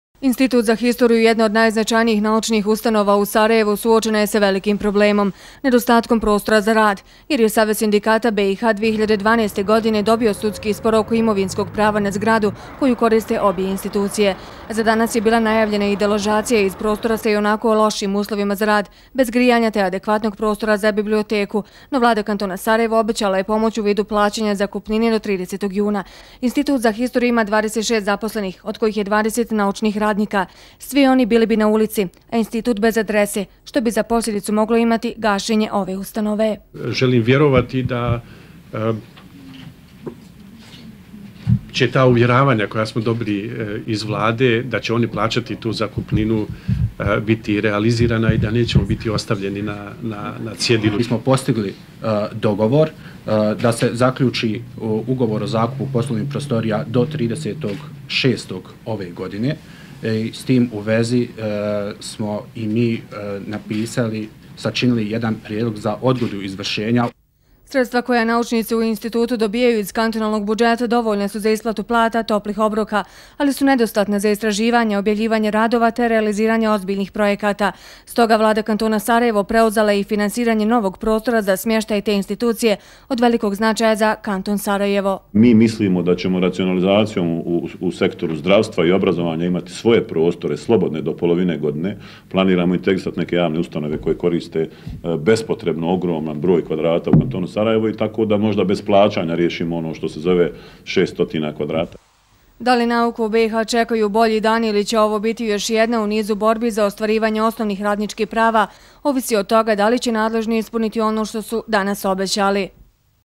Institut za istoriju za sada ostaje u zgradi u Alipašinoj ulici (FTV)
Deložacija Instituta za historiju iz zgrade koju koristi od 1962, iako je bila najavljena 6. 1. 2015. u 11:00, nije izvedena, jer je postignut dogovor sa Savezom samostalnih sindikata Bosne i Hercegovine. Vlada Kantona Sarajevo je obećala plaćati zakupninu narednih šest mjeseci, a premijer Elmedin Konaković je u ime Vlade obećao u tom razdoblju naći trajno rješenje za smještaj Instituta.